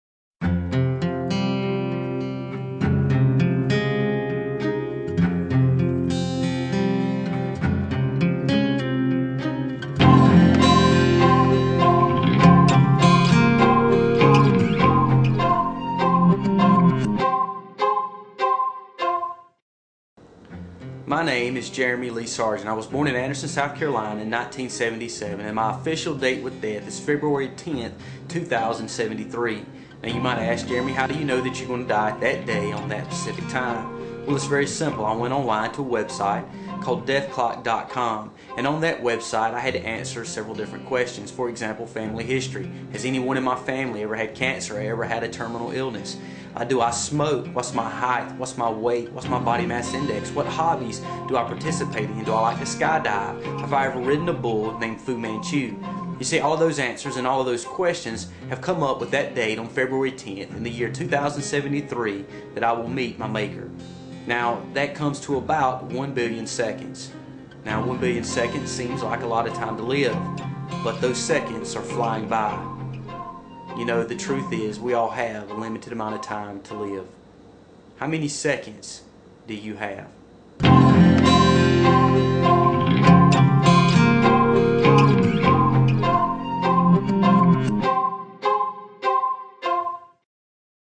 30 Days to Live Sermon Intro
Sermon intro for "Eternal Perspectives." How many seconds do you have to live.